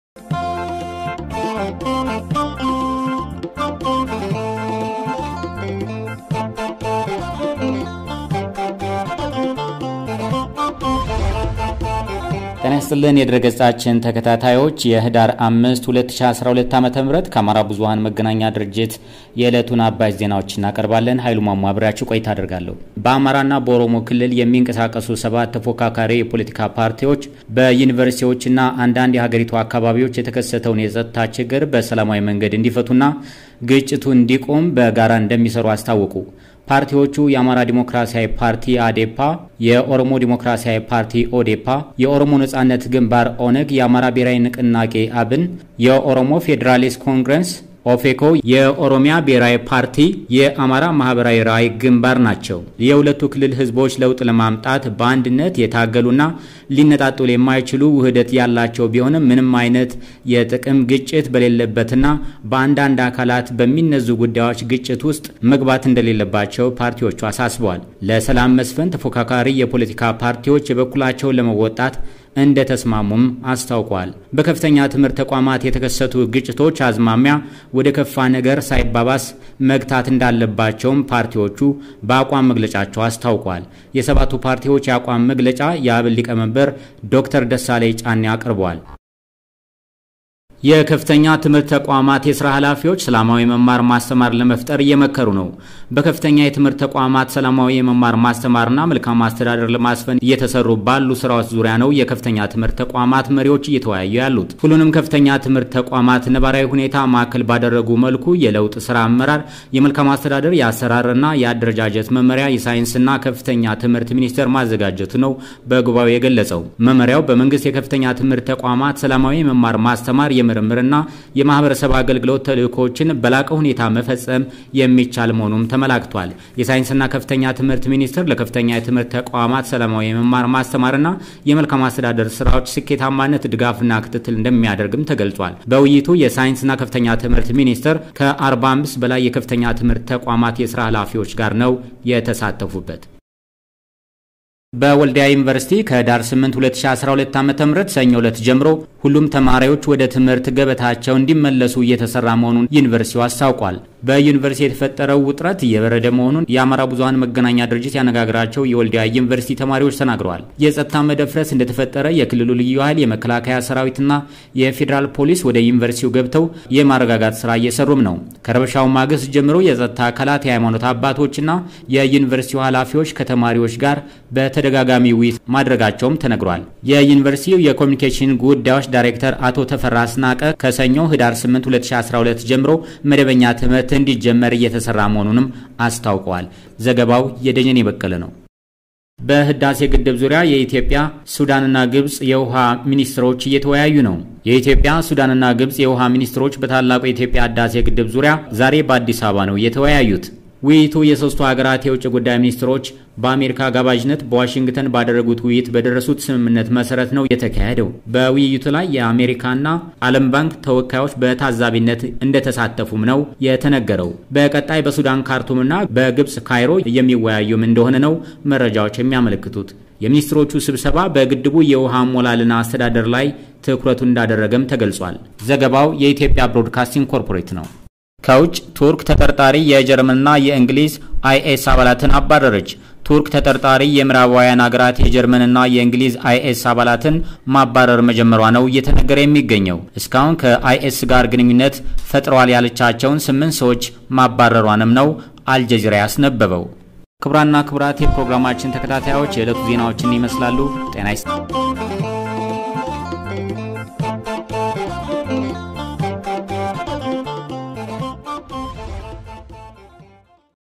በአማራና ኦሮሚያ ክልል የሚንቀሳቀሱ ፓርቲዎች በዩኒቨርሲቲዎች አካባቢ የሚስተዋለው ግጭት በሠላማዊ መንገድ እንዲፈታ ማሳሰባቸውና ሌሎች ዋና ዋና ዜናዎች ይዘናል
በአማራና ኦሮሚያ ክልል የሚንቀሳቀሱ ፓርቲዎች በዩኒቨርሲቲዎች አካባቢ የሚስተዋለው ግጭት በሠላማዊ መንገድ እንዲፈታ ማሳሰባቸውና ሌሎች ዋና ዋና ዜናዎችን ይዘናል